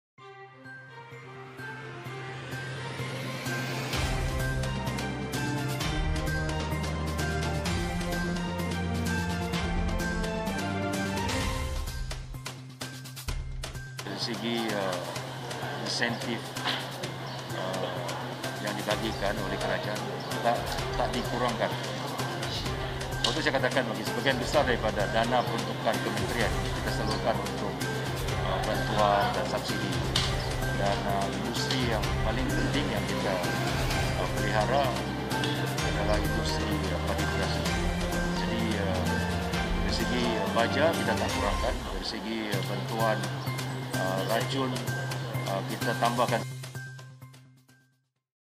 MAFI Peruntukan RM62juta insentif racun kepada pesawah
Kementerian Pertanian dan Industri Makanan (MAFI) memperuntukkan sebanyak RM62 juta untuk insentif racun kepada pesawah tahun ini.

Menterinya, Datuk Seri Dr Ronald Kiandee berkata, ia merupakan peruntukan tambahan 100 ringgit kepada RM200 yang diterima pesawah sebelum ini sekaliguskan memanfaatkan lebih 300,000 pesawah.

Beliau berkata demikian pada sidang media selepas Majlis Pelancaran Logo Sambutan 50 Tahun Lembaga Kemajuan Pertanian Kemubu (KADA) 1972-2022 di Ladang Merdeka, di Pasir Puteh.

MAFI antara kementerian yang menerima
peruntukan tinggi daripada kerajaan tahun ini iaitu sebanyak RM4.82 bilion

Sebahagian besar dari peruntukan itu akan digunakan untuk penyaluran subsidi termasuk subsidi padi beras.